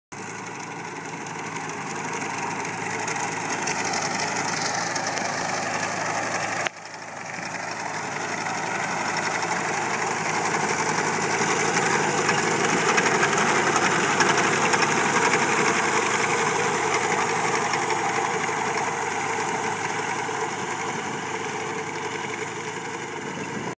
Massey Harris Pony with sickle bar mowing
Here is Cozmo Cramer mowing 40 acres on his Massey Harris Pony with its 60" Sickle Bar mower. This is a 1947 12 horse power tractor with hydraulic lift. Cozmo Cramer is a mowing machine!